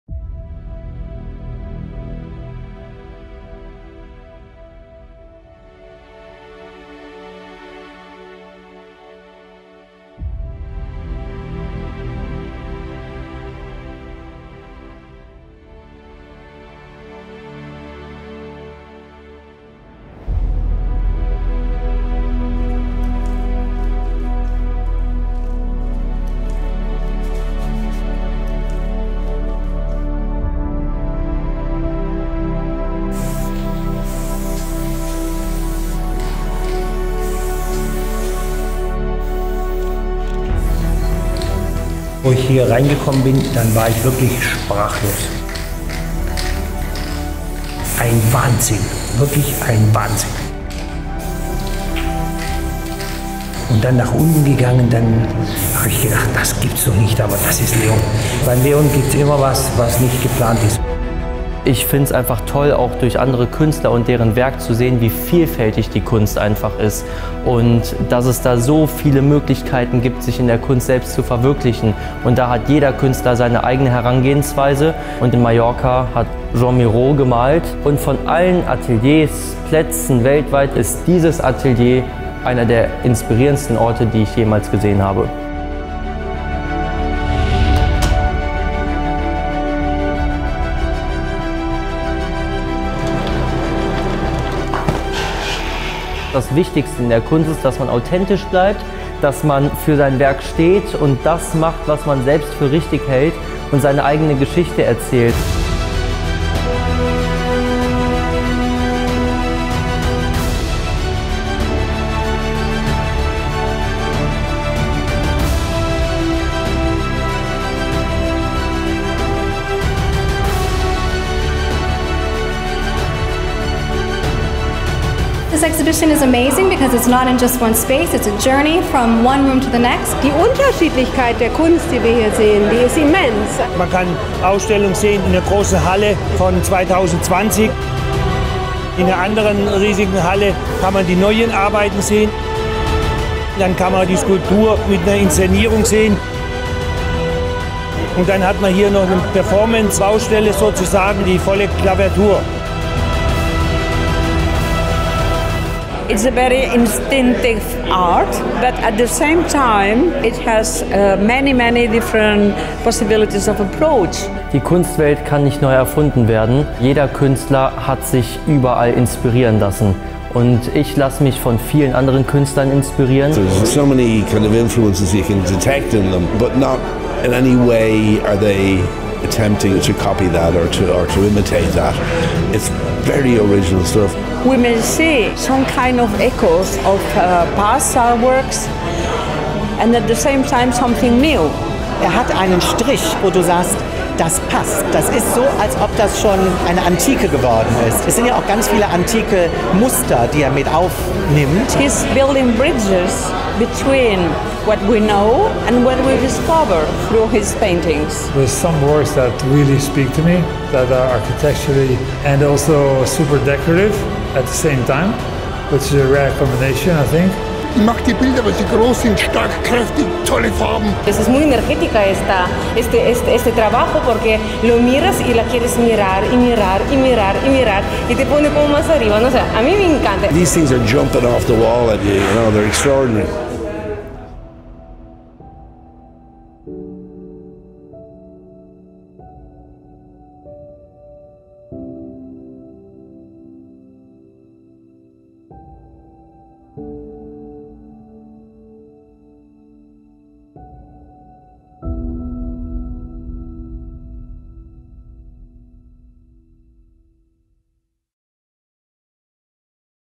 Leon Löwentraut Palma Exhibition @ Gerhardt Braun Gallery
Vernissage of Leon Löwentraut at Gerhardt Braun Gallery, Palma de Mallorca, Spain, 2021